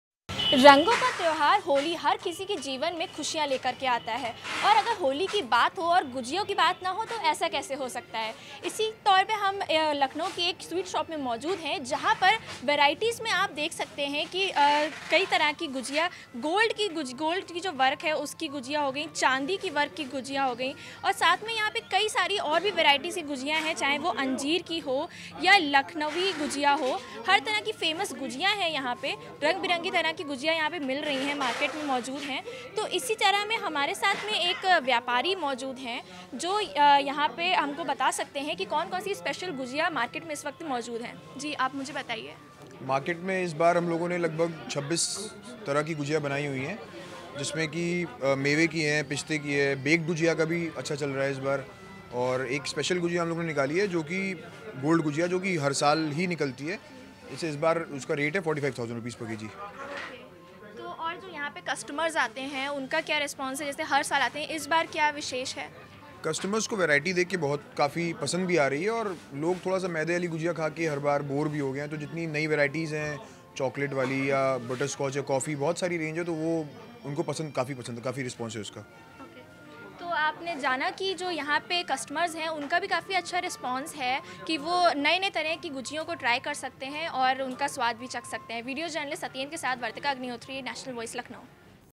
National Voice: इस होली खाएं ये खास गुझिया
कुछ ही दिनों बाद होली का त्योहार दस्तक देने वाला है. रंगों का यह त्योहार एक खास चीज के बिना अधूरा सा माना जाता है, और वो चीज है गुझिया.